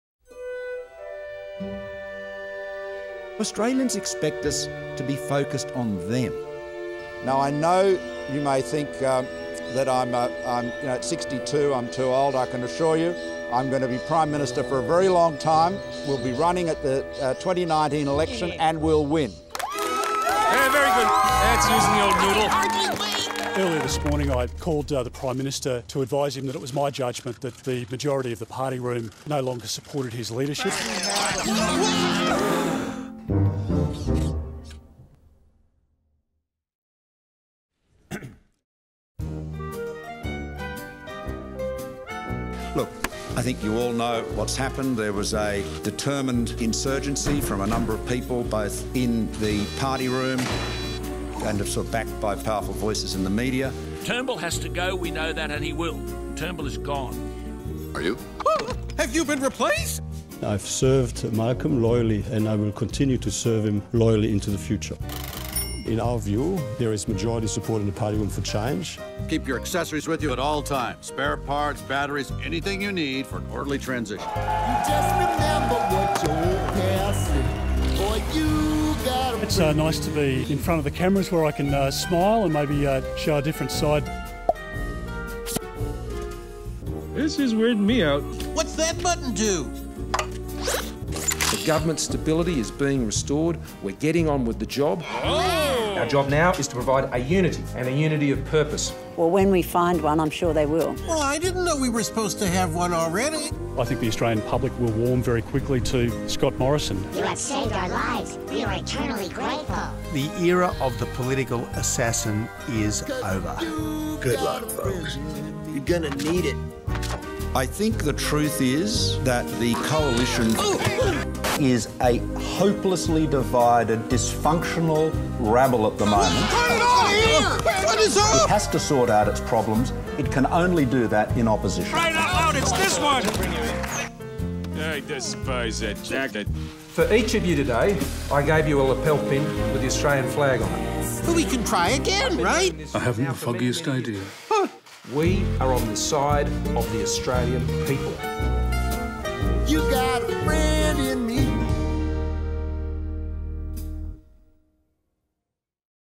Tory Story
Leadership troubles can be tough, even in the toy world.